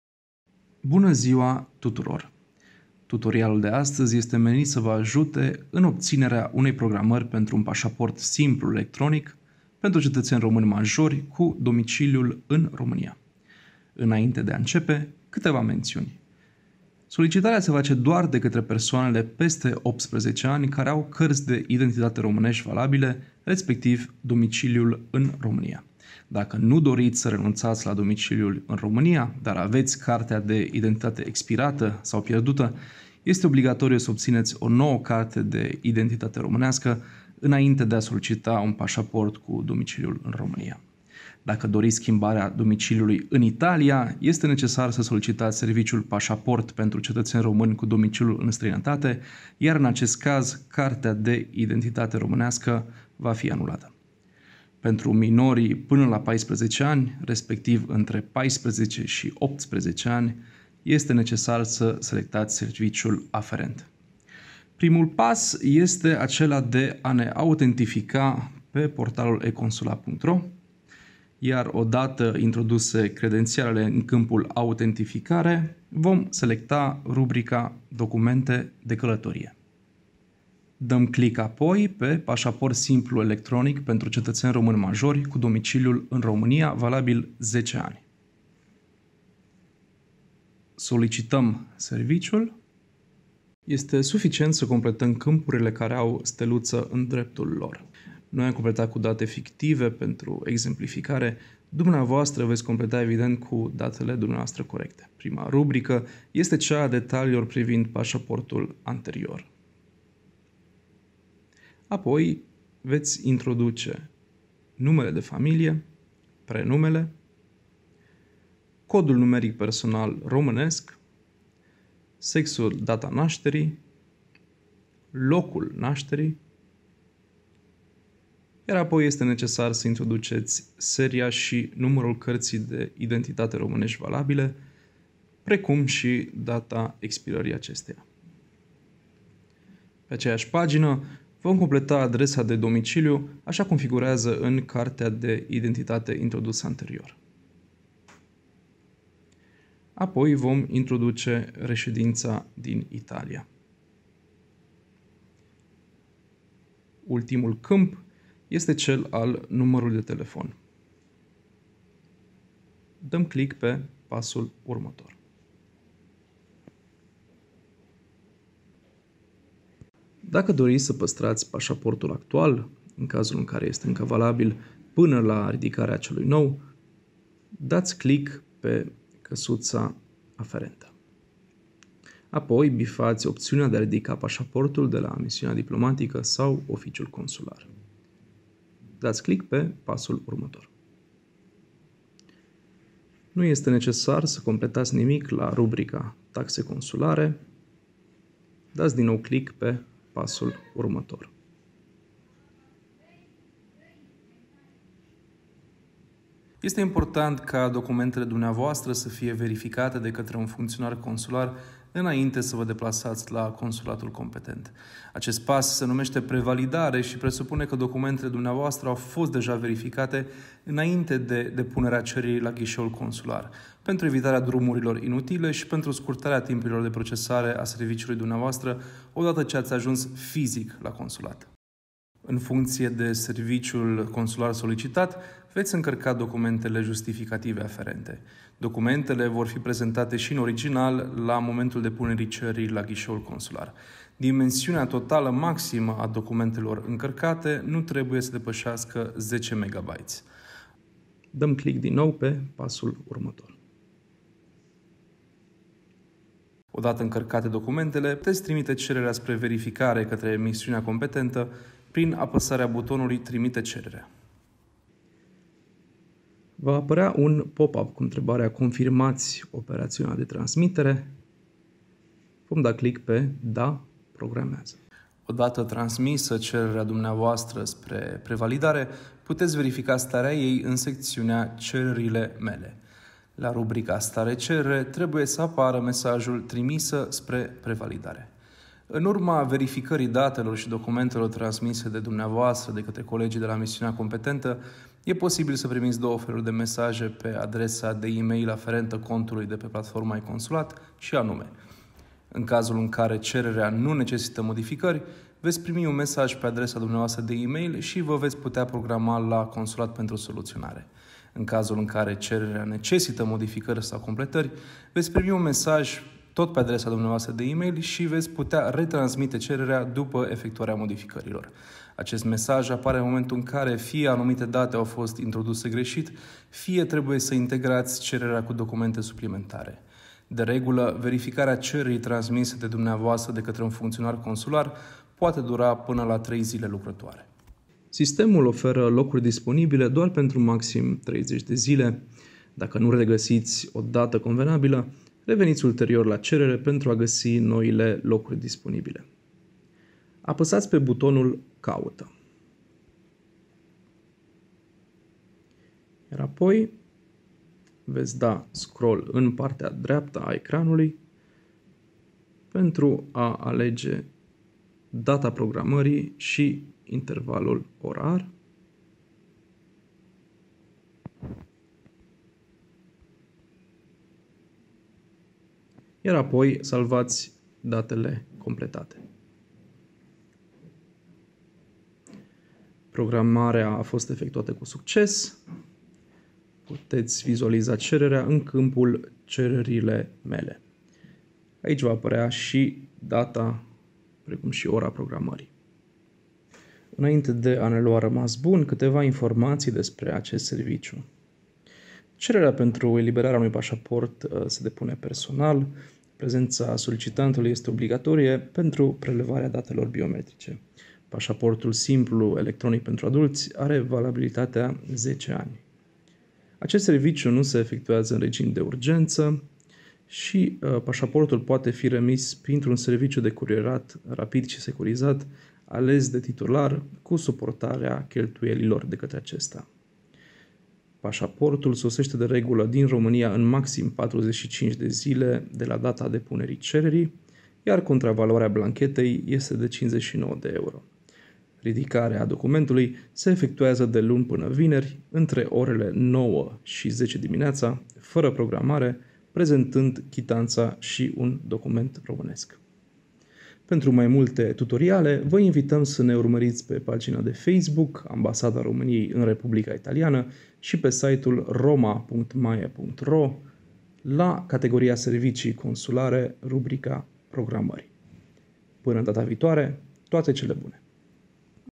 GHID - Programare pașaport electronic cu domiciliul în România (Smartphone)
Dacă aveți nevoie de pașaport și nu știți cum puteți să obțineți o programare pentru acest serviciu consular vă punem la dispoziție următorul ghid care explică, în câțiva pași simpli, modalitatea prin care puteți obține aceasta programare, în numai câteva minute, direct de pe telefonul dumneavoastră.

Nu uitați, programările pentru serviciile consulare sunt GRATUITE!
Nu apelați la serviciile unor persoane sau societăți de consultanță care pretind a fi colaboratori ai Secției Consulare sau ale Consulatelor României. Puteți fi victime ale infracțiunii de înșelăciune și furt de identitate.